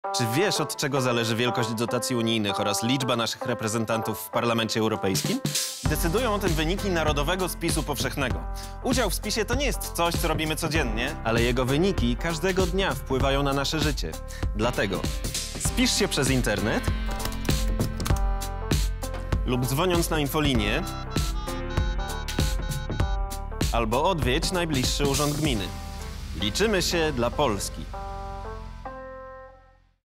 Wpływ spisu
Narodowy Spis Powszechny Ludności i Mieszkań trwa od 1 kwietnia do 30 września 2021 r.
Udział w spisie to nie jest coś, co robimy codziennie, ale jego wyniki każdego dnia wpływają na nasze życie.
Spisz się przez Internet (💻 spis.gov.pl) lub dzwoniąc na infolinię (☎️ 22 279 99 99).